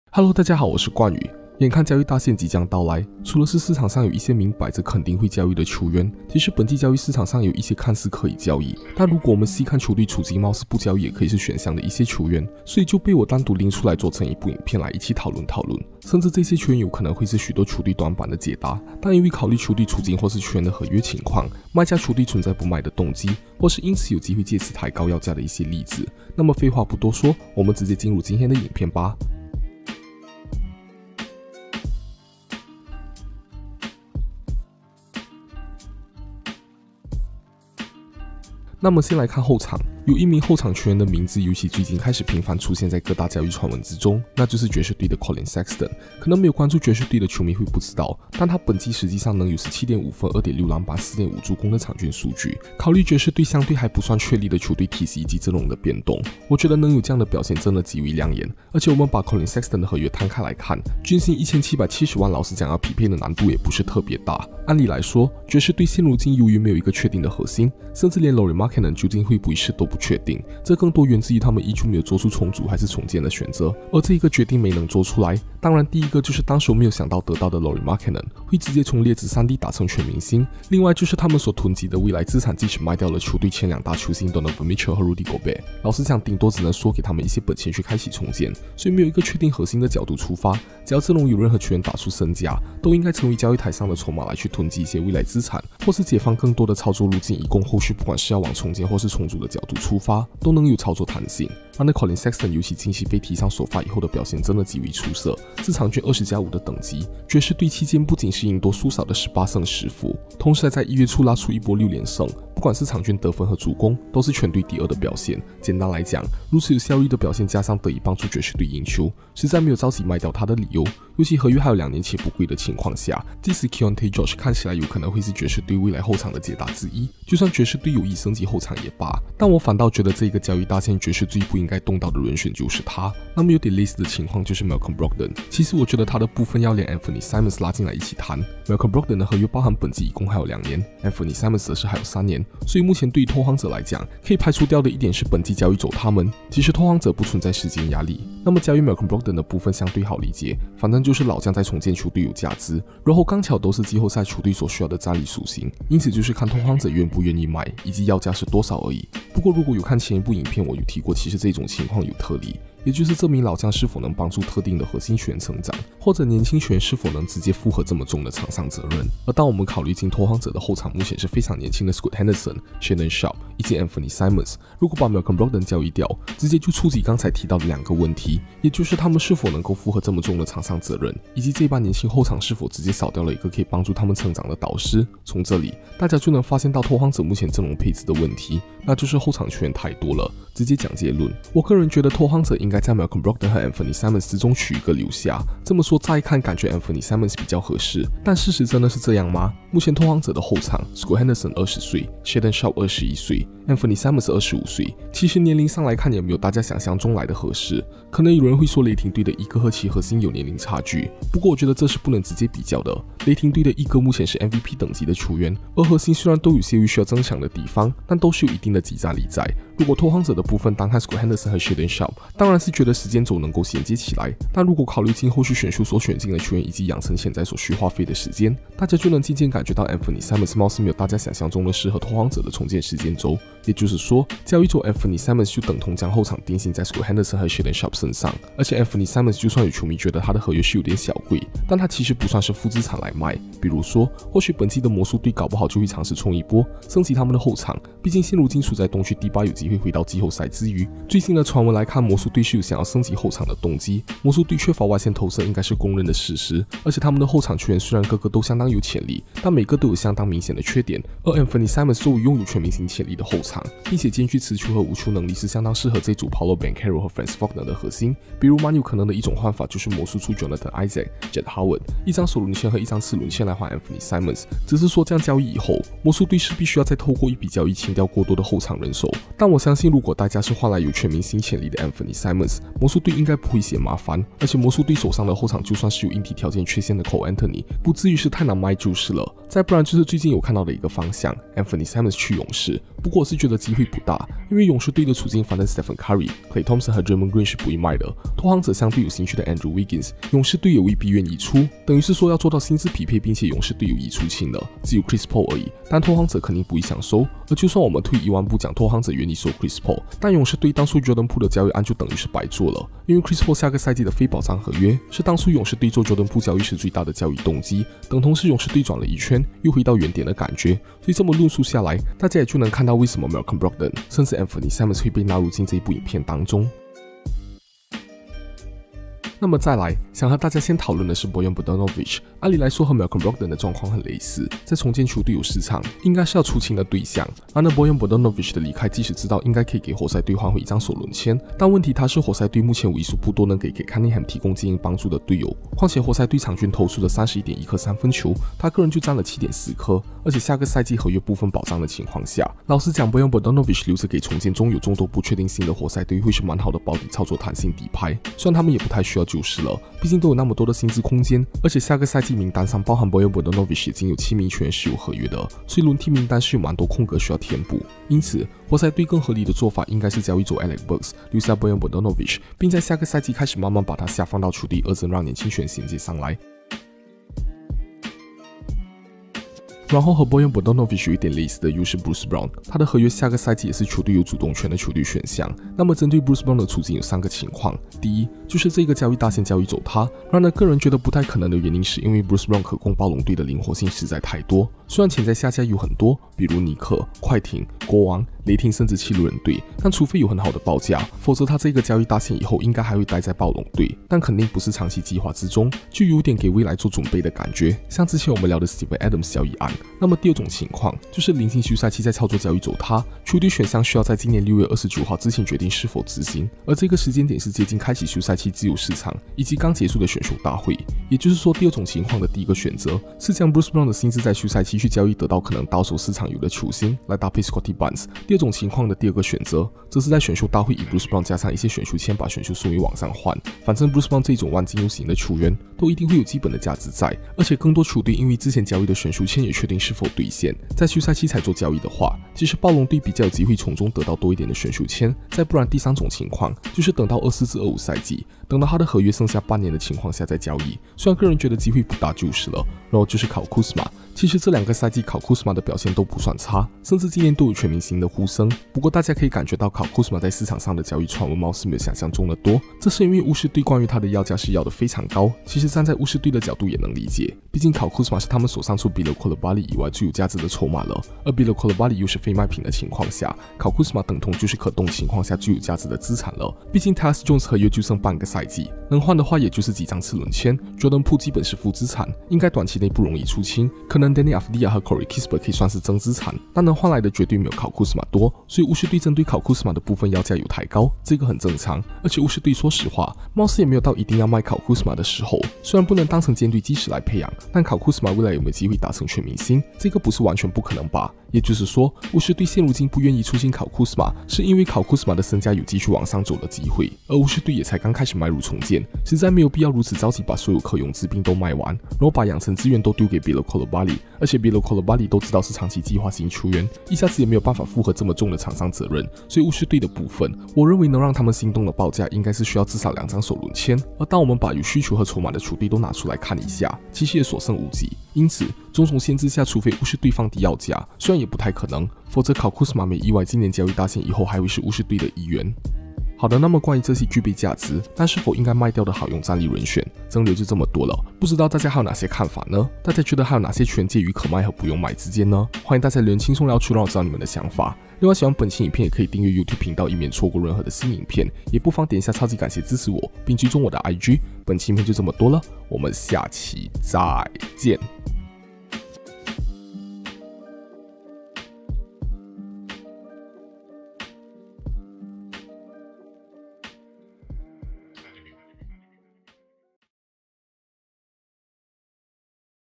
NBA 🏀 具備市場價值，但需要交易嗎？本季交易大限是否應該賣掉這些好用戰力？（冠祤聊球）
眼看NBA交易大限即將到來，除了是市場上有一些明擺著肯定會交易的球員，其實本季交易市場上也有一些看似可以交易，但如果我們細看球隊處境貌似不交易也可以是選項的一些球員。所以就單獨拎出來做成一部影片來一起討論討論。甚至這些球員有可能會是許多球隊短板的解答，但因為考慮球隊處境或是球員的合約情況，賣家球隊存在不賣的動機，或是因此有機會借此擡高要價的一些例子。